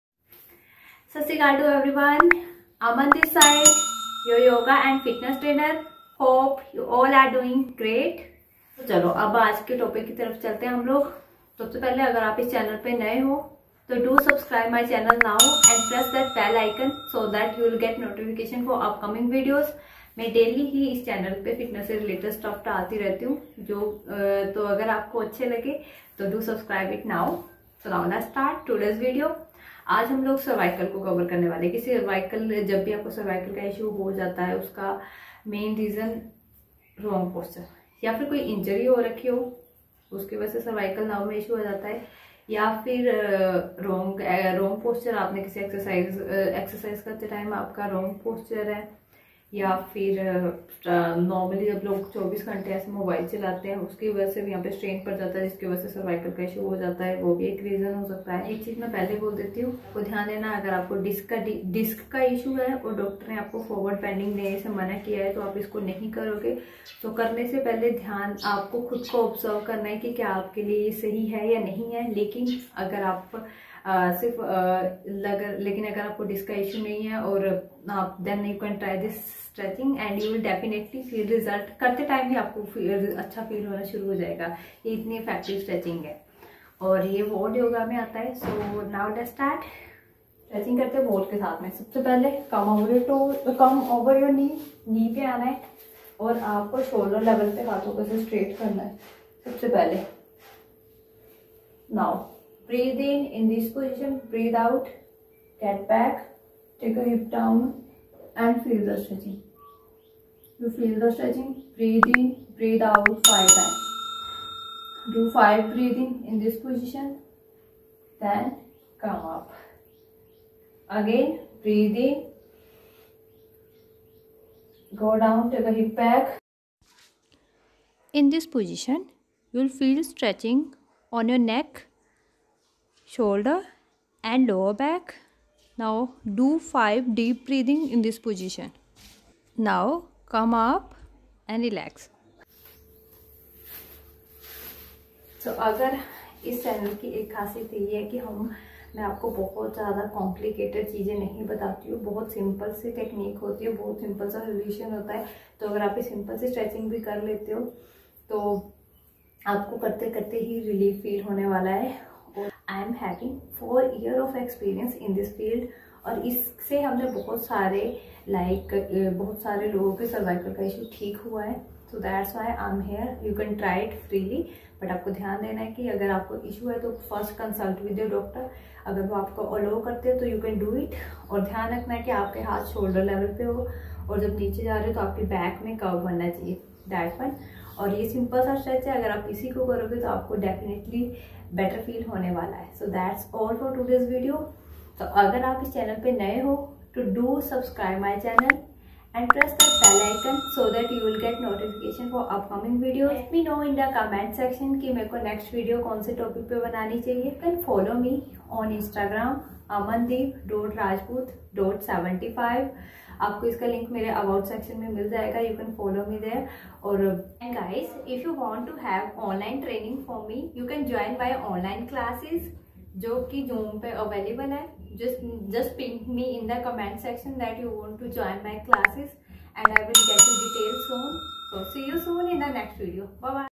5 minutes wall strech for CERVICAL PAIN! MUST TRIED
Hey guys, Aman this side your yoga and fitness trainer in this vedio I am showing strech for cervical pain. But before try this just consult with your doctor in case of any injury or disc issue. This is easy and will take only  5 minutes strech. You will feel better after doing it. While doing this just be focused on breathing have effortless and smooth inhalation and exhalation.

Hope this will help someone do share 🙏with your friends and family

You can follow me on instragram
Username: Amandeep.rajpoot.75

 For any online training related queries